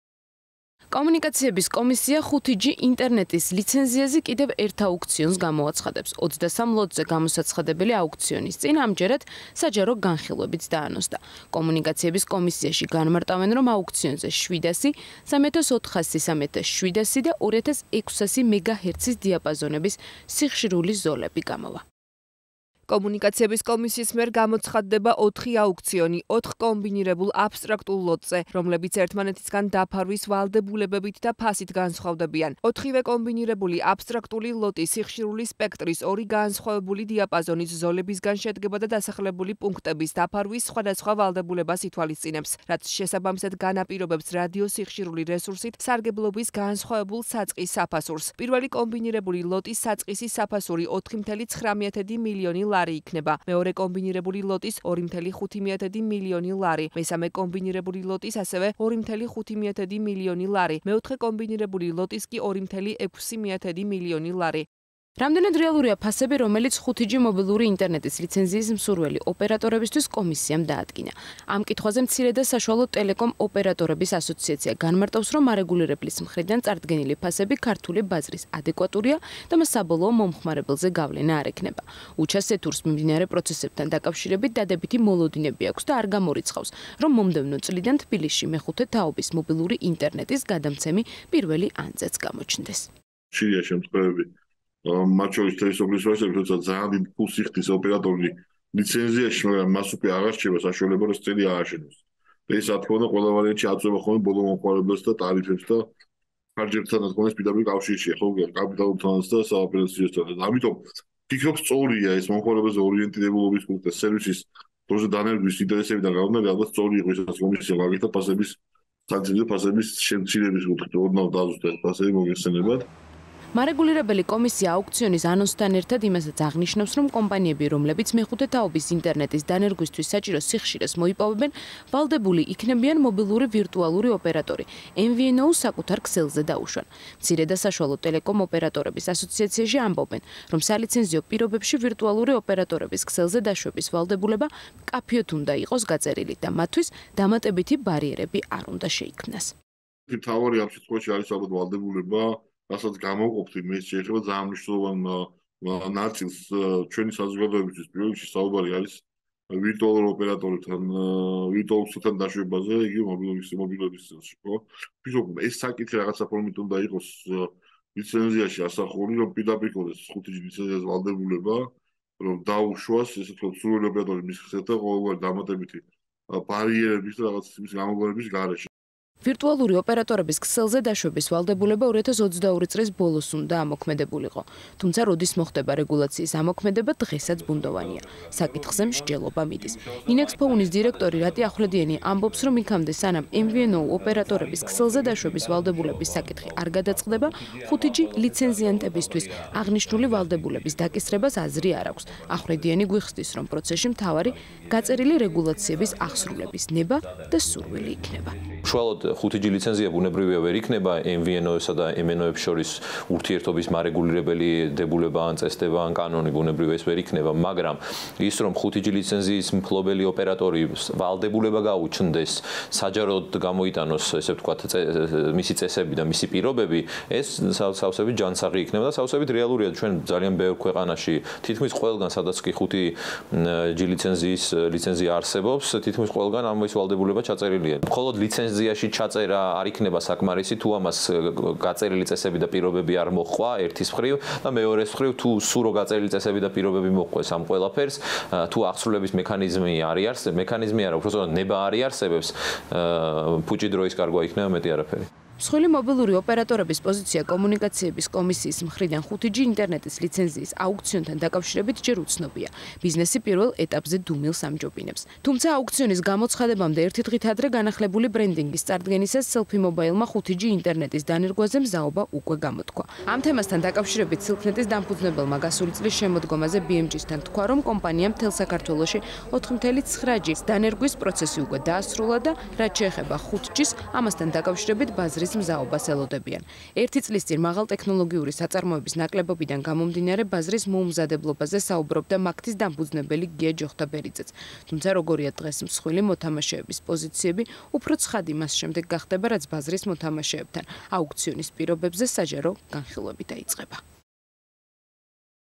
კომუნიკაციების კომისია 5G-ის დასანერგად აუქციონს გამოაცხადებს;
მორიგი მცდელობა ქვეყანაში 5G მობილური ინტერნეტის აუქციონის ჩასატარებლად - კომუნიკაციების კომისია საჯარო კონსულტაციებს იწყებს. კომისიის ინფორმაციით, საჯარო კონსულტაციების მიზანია აუქციონის გამოცხადებამდე ყველა დაინტერესებულ პირს მიეცეს შესაძლებლობა, წარმოადგინოს საკუთარი მოსაზრებები აუქციონის პირობებთან დაკავშირებით. რამდენად რეალურია ფასები, რომელიც 5G მობილური ინტერნეტის ლიცენზიის მსურველი ოპერატორებისთვის დაადგინა? ამ კითხვების დასმა პირველ რიგში სატელეკომუნიკაციო ბაზრის მსხვილ მოთამაშეებთან ვცადეთ, თუმცა მათ მარეგულირებლის წარდგენილ სააუქციონე პირობებზე კომენტარის გაკეთება ამ ეტაპზე არ ისურვეს. „მცირე და საშუალო ტელეკომ ოპერატორების ასოციაციის“ ხელმძღვანელი უჩა სეთური ამბობს, რომ მარეგულირებლის მხრიდან წარდგენილი ფასები ქართული ბაზრის ადეკვატურია და მას საბოლო მომხმარებელზე გავლენა არ ექნება.